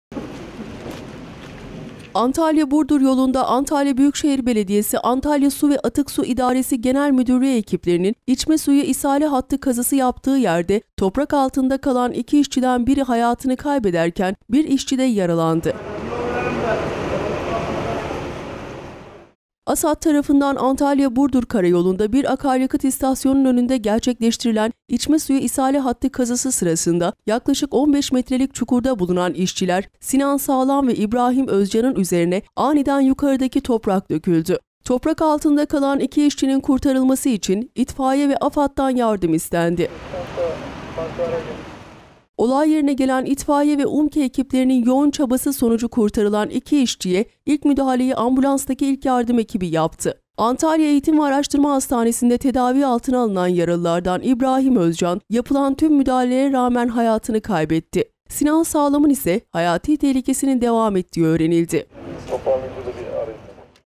KAZI YAPAN İŞÇİLER TOPRAK ALTINDA KALDI
Antalya-Burdur yolunda, Antalya Büyükşehir Belediyesi Antalya Su ve Atıksu İdaresi Genel Müdürlüğü ekiplerinin içme suyu isale hattı kazısı yaptığı yerde toprak altında kalan 2 işçiden biri hayatını kaybederken bir işçi de yaralandı.
ASAT tarafından Antalya-Burdur karayolunda bir akaryakıt istasyonunun önünde gerçekleştirilen içme suyu isale hattı kazısı sırasında yaklaşık 15 metrelik çukurda bulunan işçiler Sinan Sağlam ve İbrahim Özcan'ın üzerine aniden yukarıdaki toprak döküldü.
Toprak altında kalan iki işçinin kurtarılması için itfaiye ve AFAD'dan yardım istendi.
Olay yerine gelen itfaiye ve UMKE ekiplerinin yoğun çabası sonucu kurtarılan iki işçiye ilk müdahaleyi ambulanstaki ilk yardım ekibi yaptı.
Antalya Eğitim ve Araştırma Hastanesi'nde tedavi altına alınan yaralılardan İbrahim Özcan yapılan tüm müdahalelere rağmen hayatını kaybetti. Sinan Sağlam'ın ise hayati tehlikesinin devam ettiği öğrenildi.
Öte yandan olay anı akaryakıt istasyonunun güvenlik kameralarına yansıdı.